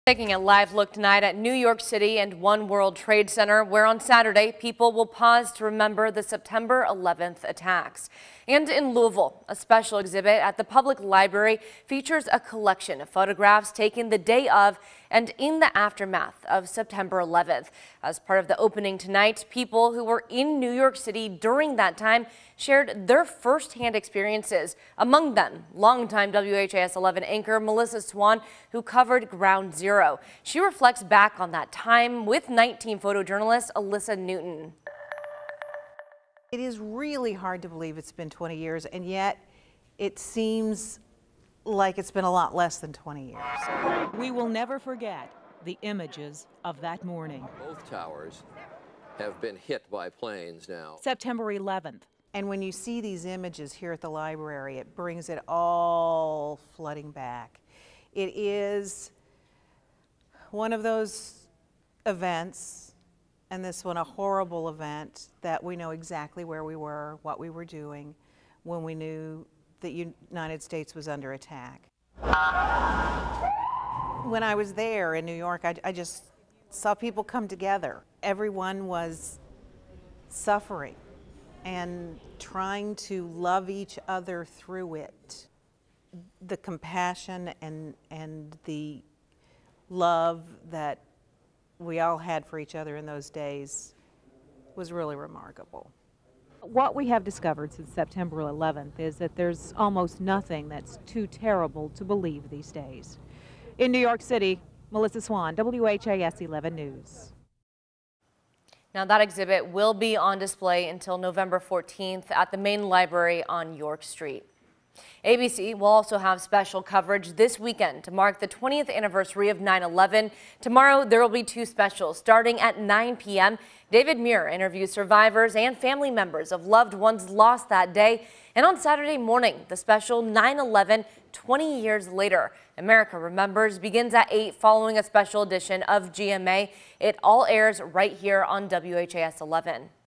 Library exhibit features collection of photos from September 11
In Louisville, a special exhibit at the public library features a collection of photographs taken the day of and in the aftermath of September 11.